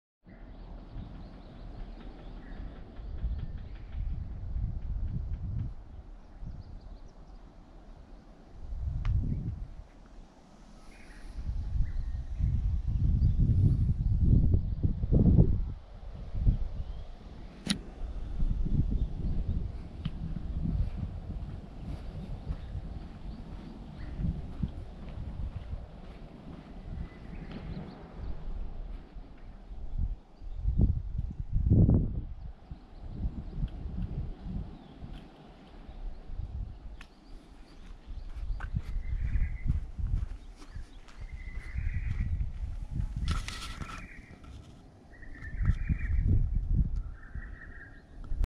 11438 針ヶ別所町戸建（706） 外観
奈良市針ケ別所町にあります5LDK戸建ての【針ヶ別所町戸建】の紹介動画になります。